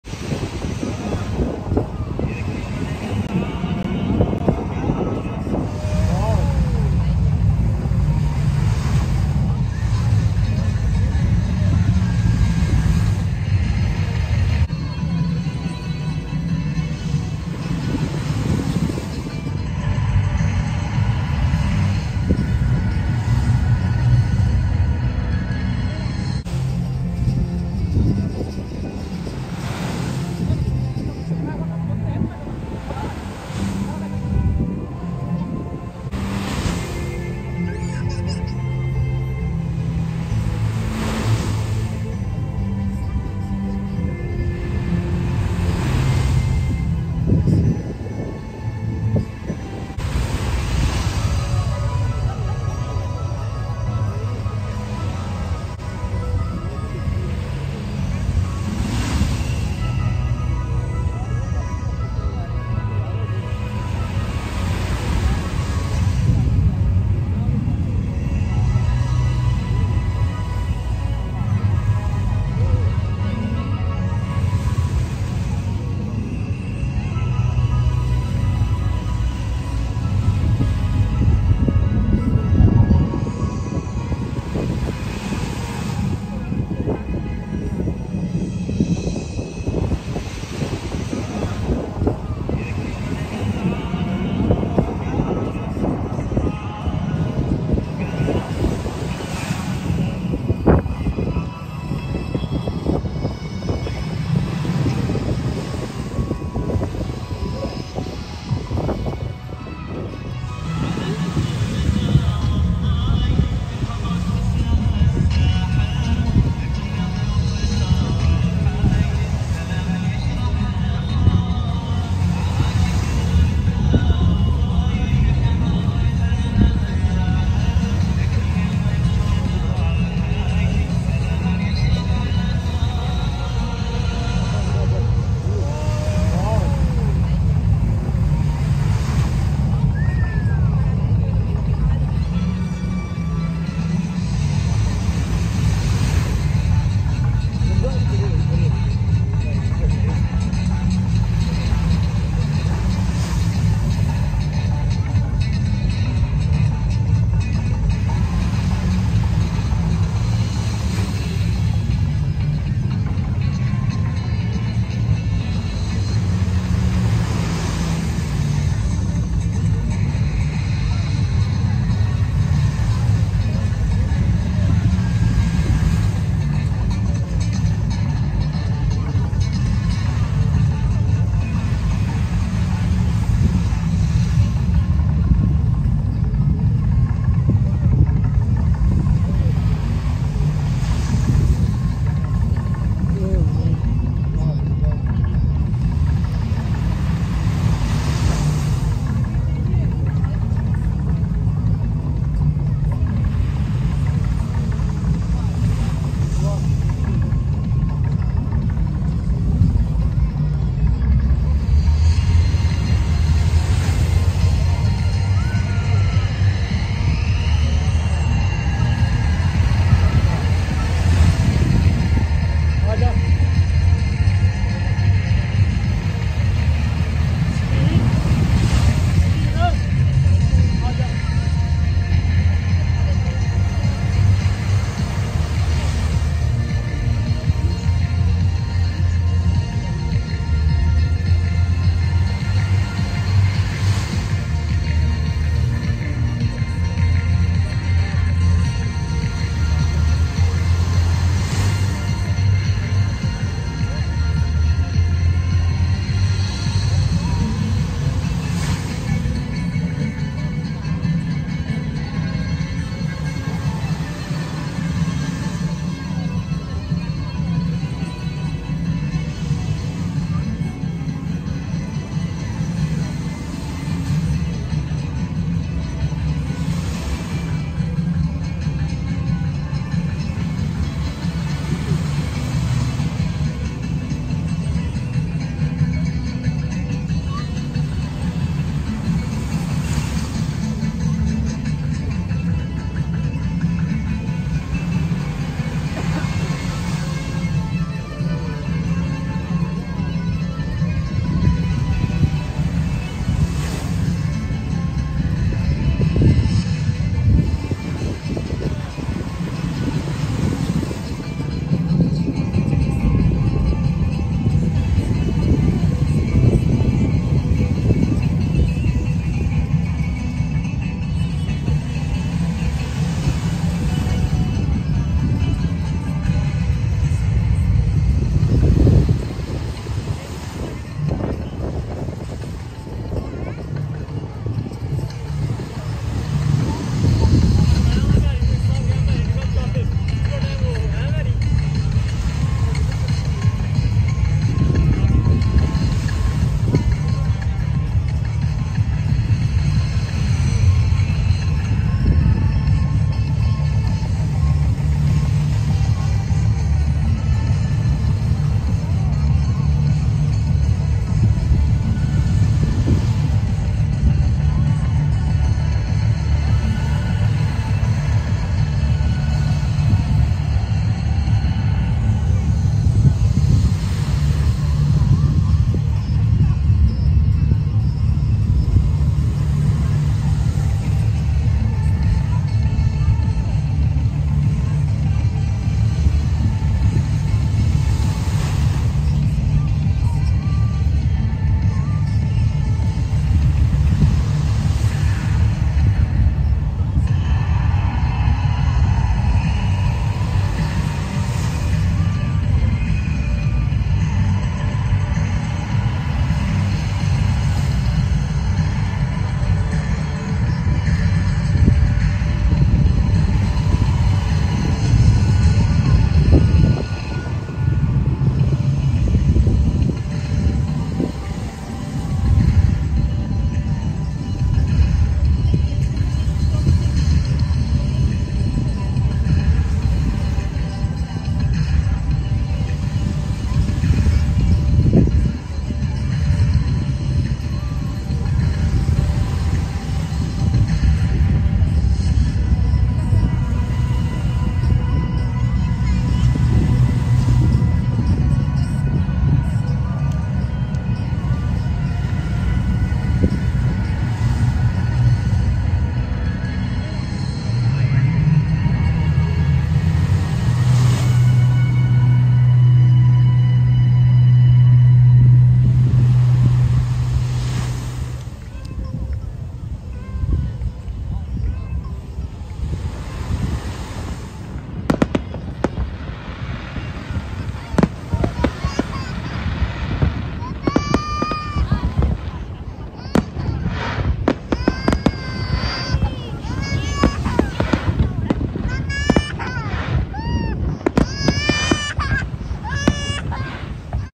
Dubai drones show | DSF Drones Light Show | Dubai Shopping Festival
The longest-running drone light show in the region returns with twice-nightly performance live from Bluewaters and The Beach, JBR
Everyday at 7pm & 9:30 pm during the 28th edition of Dubai Shopping Festival (DSF) from 15 December 2022 to 29 January 2023.